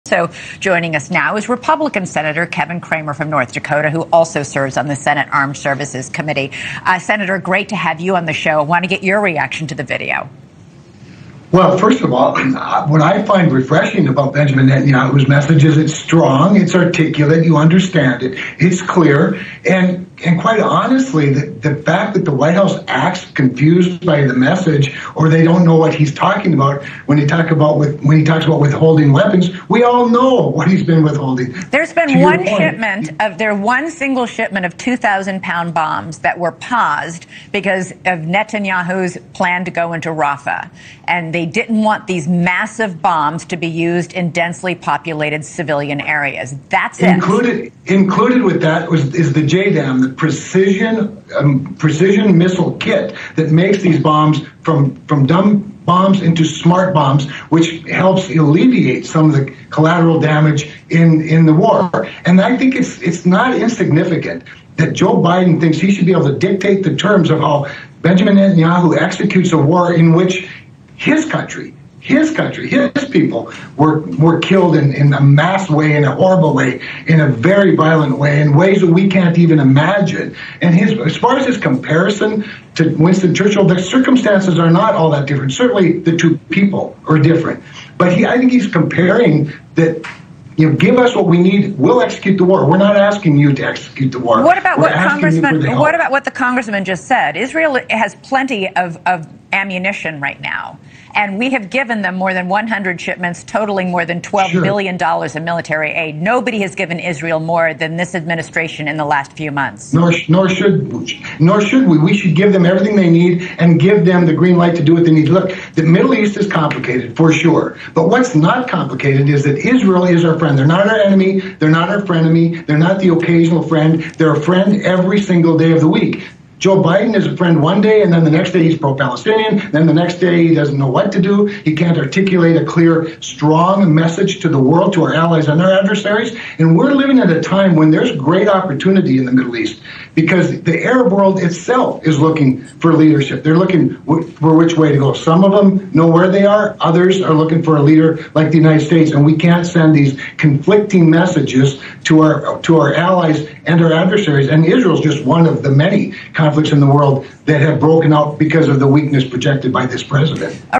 Senator Cramer on NewsNation Wednesday, June 19th 2024.
"Look the Middle East is complicated for sure, but what's not complicated is that Israel is our friend. They're not our enemy, they're not our frenemy, they're not the occasional friend. They're a friend EVERY SINGLE DAY OF THE WEEK."